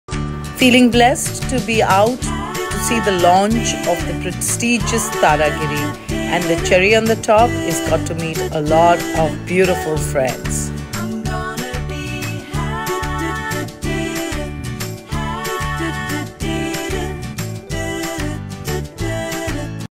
Iconic moment fortunate to be part of
Fortunate to witness the Launch of Taragiri it true iconic moment for us. Made inIndia .